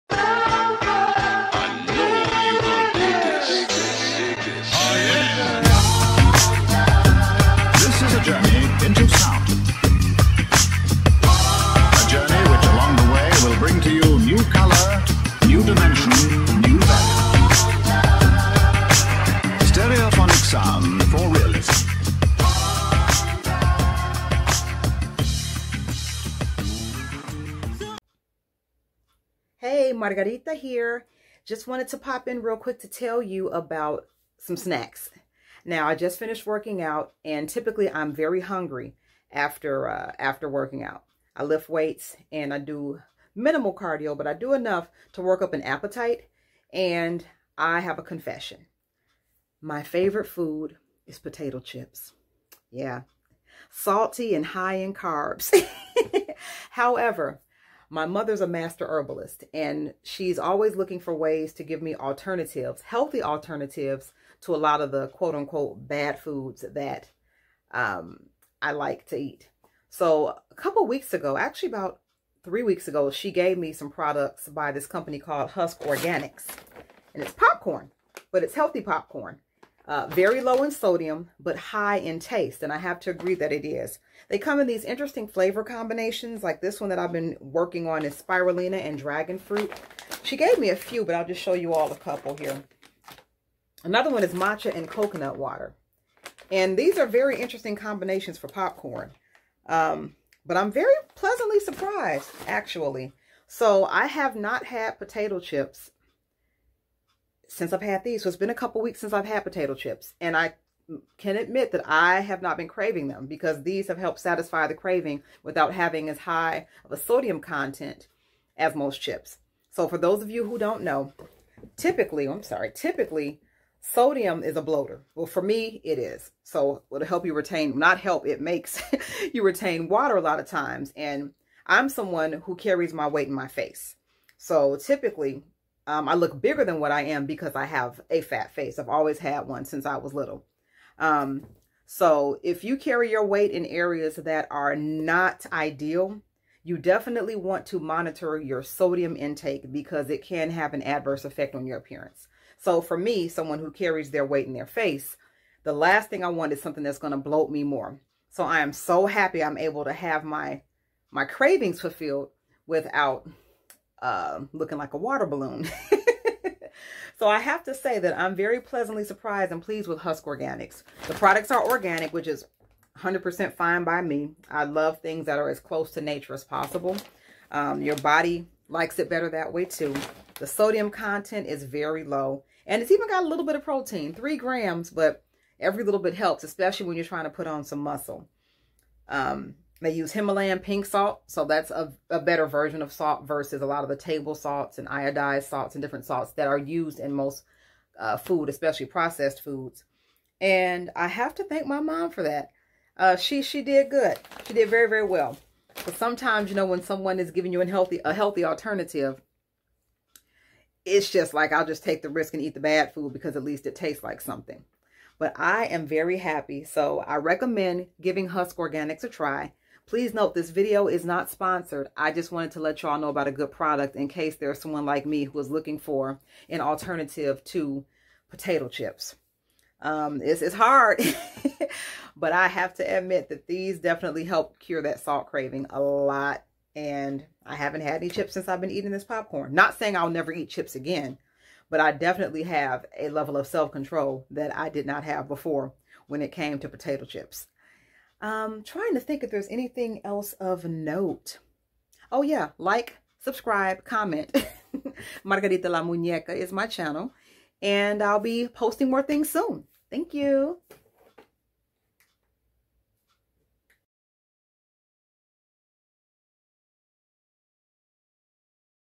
Guilt-Free Snack Recommendation
Husk Organics Popcorn- Not sponsored, I just wanted to share a great snack alternative.

 Please mail promotional items to:
 Margarita El
1507 E 53rd St, Unit 246